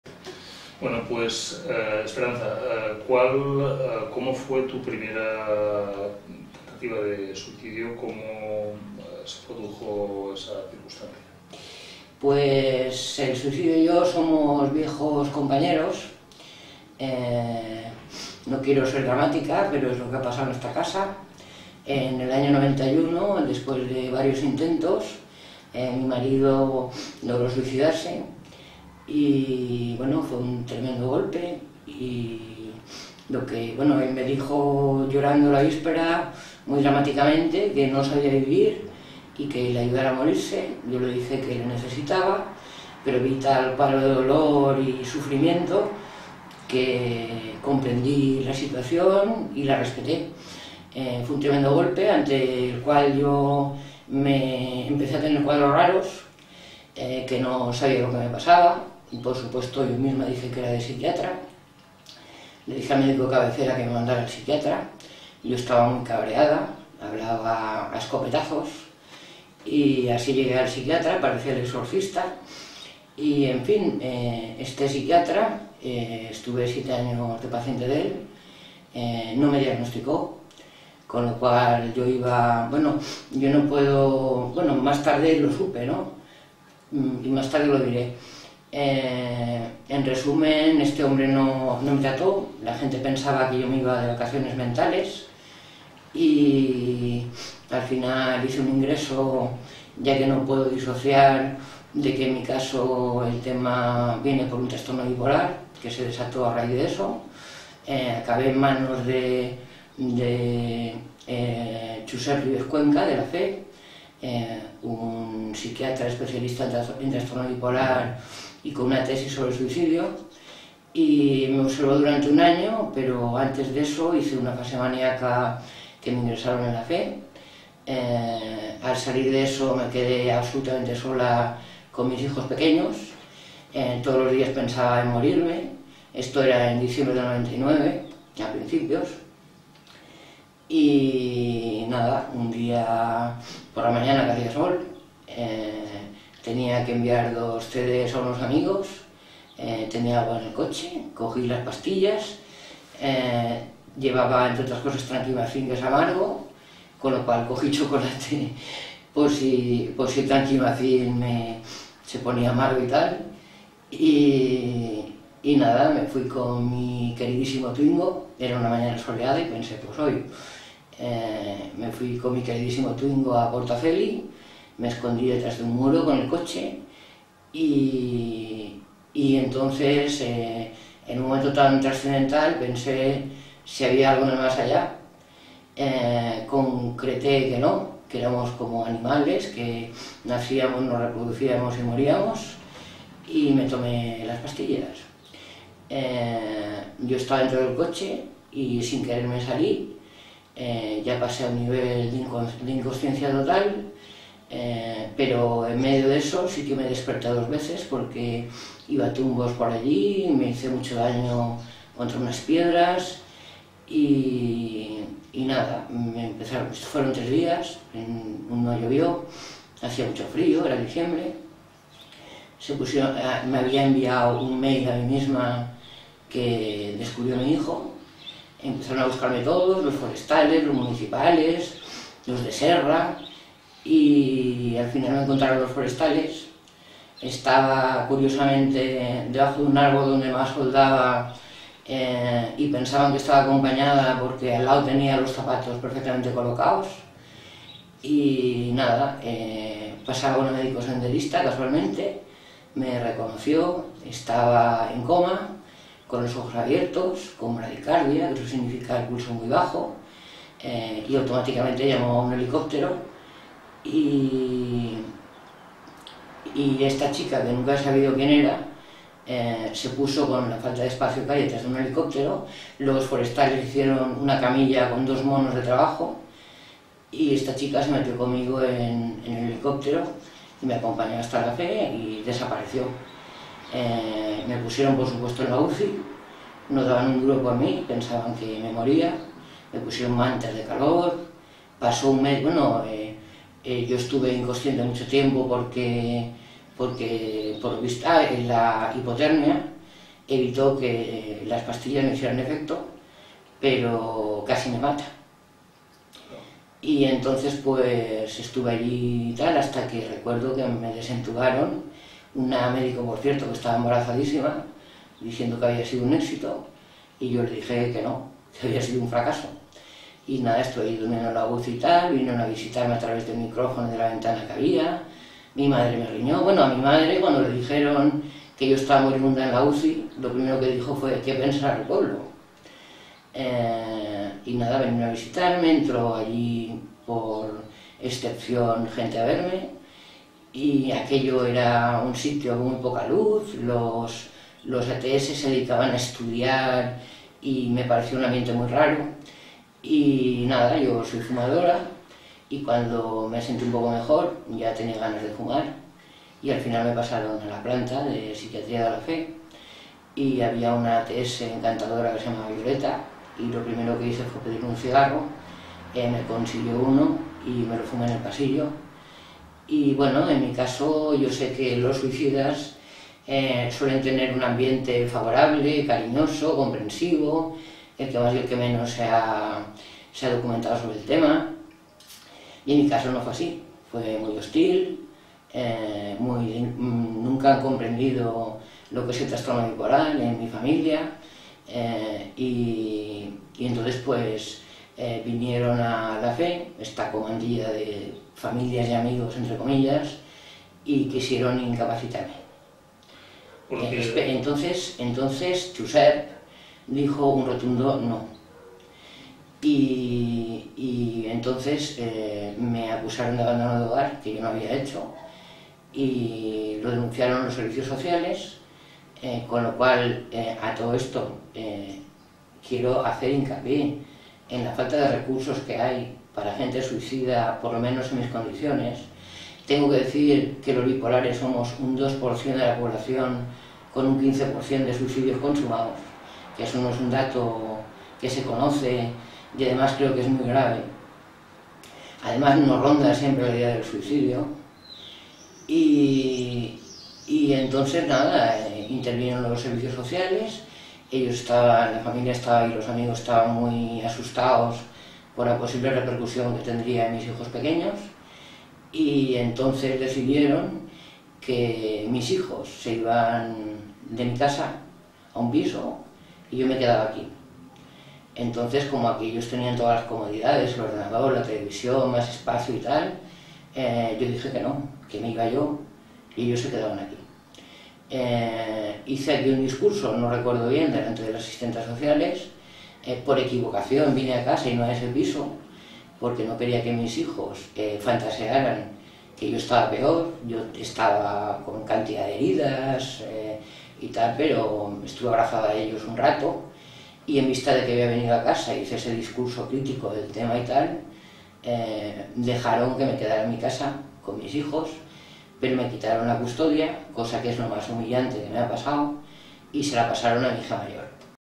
suicidio/ trastorno bipolar I
se ha publicado recientemente que el suicidio ha pasado a ser la 1ª causa de muerte no natural en españa, ahi os va un testimino para la difusión de esto tan grave, junto a otras enfermedades mentales que se desconocen